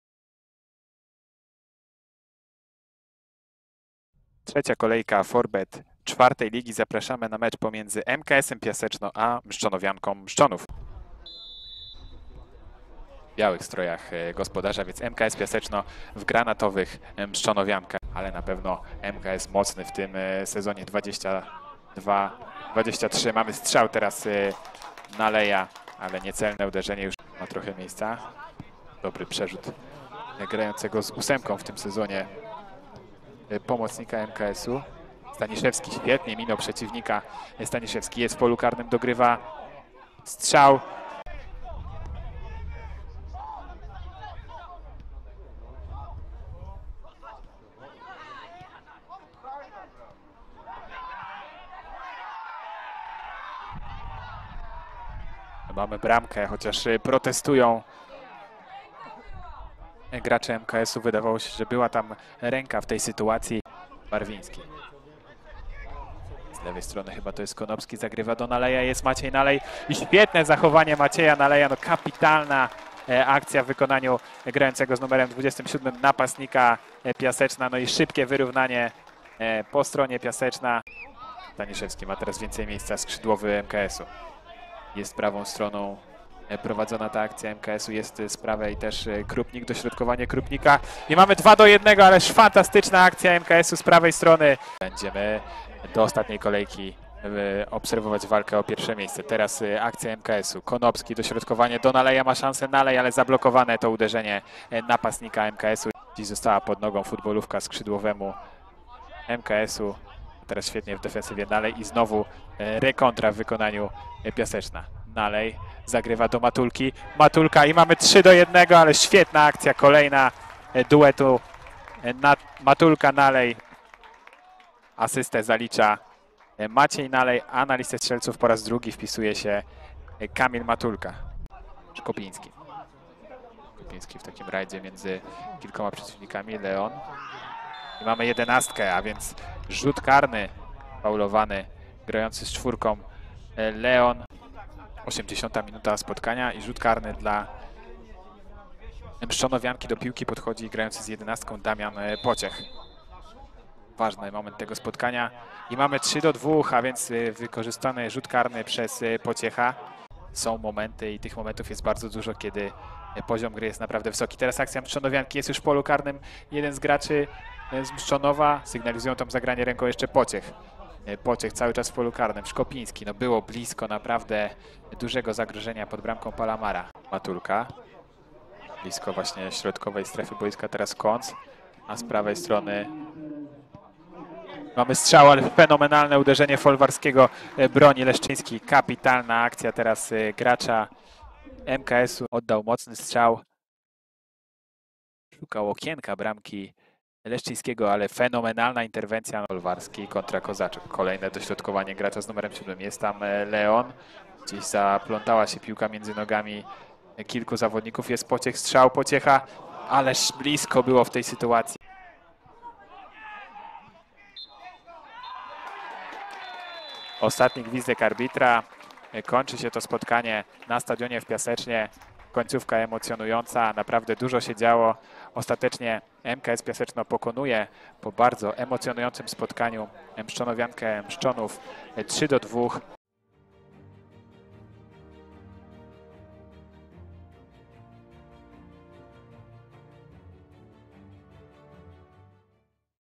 TRZECIE ZWYCIĘSTWO Z RZĘDU W NOWYM SEZONIE! Skrót meczu: MKS Piaseczno - Mszczonowianka
MKS Piaseczno vs Mszczonowianka Mszczonów,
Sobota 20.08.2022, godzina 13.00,
forBET IV liga 2022/2023, grupa: mazowiecka,
kolejka 3